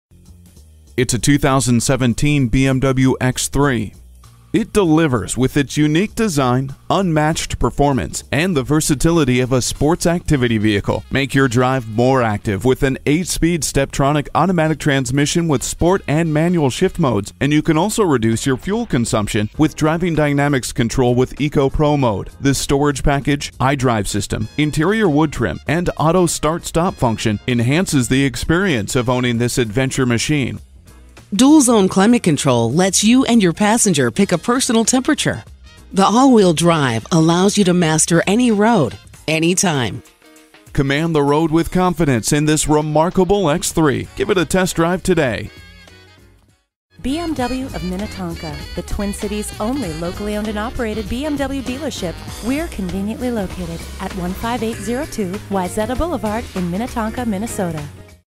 New 2017 BMW X3 Minnetonka MN Minneapolis, MN B1082 - SOLD
Year: 2017
Make: BMW
Model: X3
Trim: xDrive28i Sports Activity Vehicle
Engine: 2.0 L 4 Cyl. Cyl.
Transmission: 8-Speed Automatic
Color: Melbourne Red Metallic
Interior: Black Nevada
Stock #: B1082
VIN: 5UXWX9C50H0T05719

Address:
15802 Wayzata Blvd